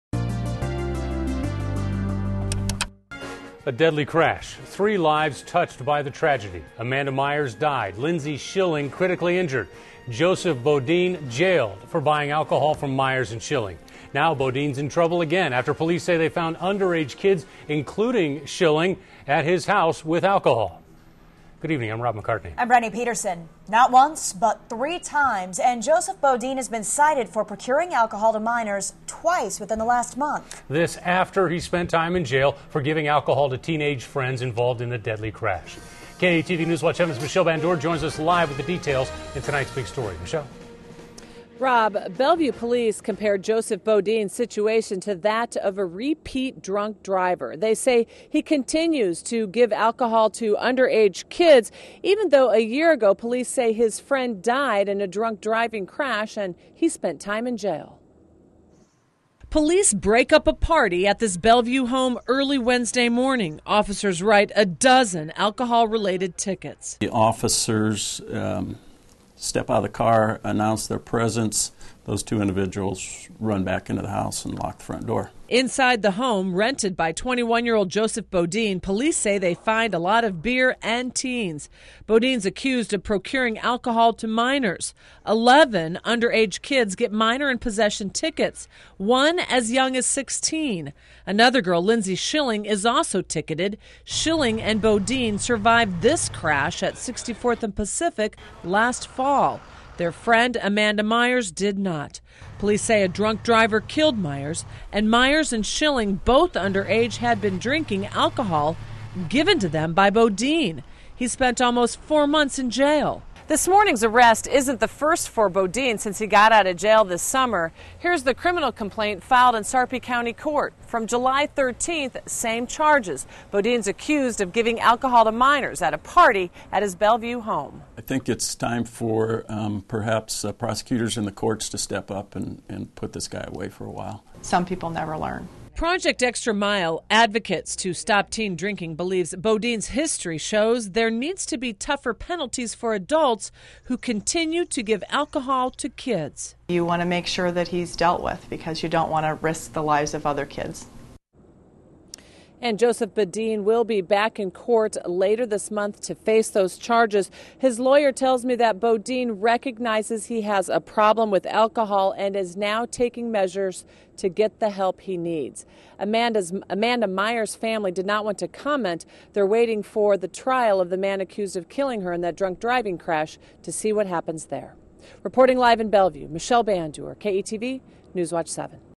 Police: Man Has History Of Giving Alcohol To Minors
A Bellevue man has been cited on suspicion of procuring alcohol to minors twice within the past month.